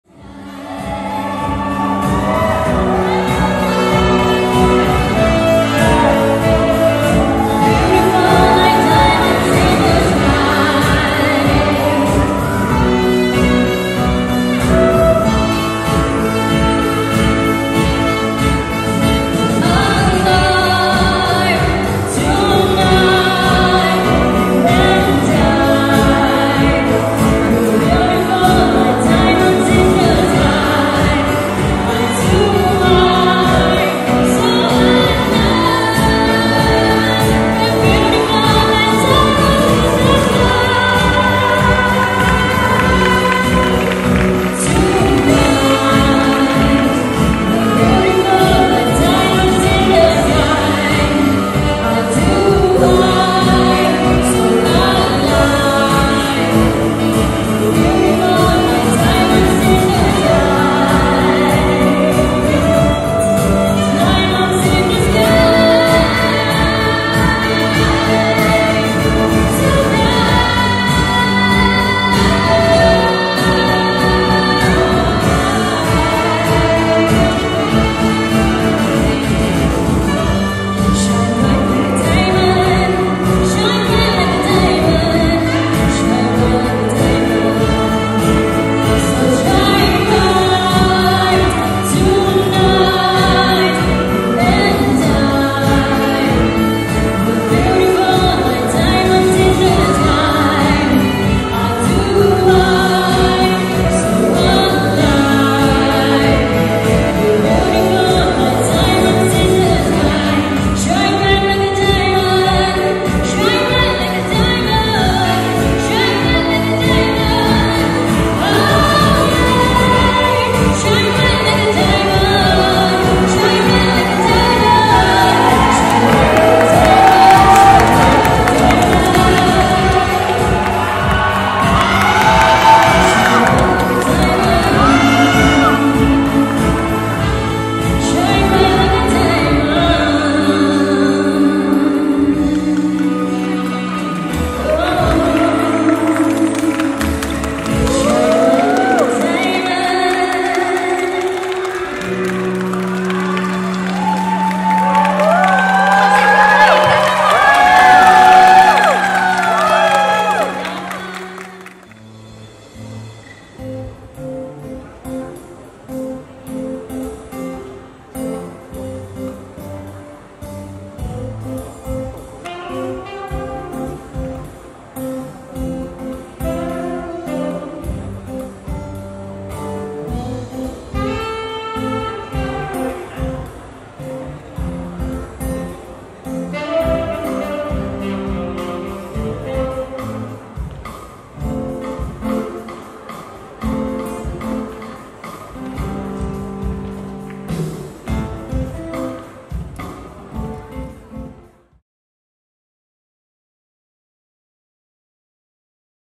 แพนเค้ก KANAPOT | Grand Opening and Fashion Show "Stand Still" Capsule Collection | VDO BY POPORY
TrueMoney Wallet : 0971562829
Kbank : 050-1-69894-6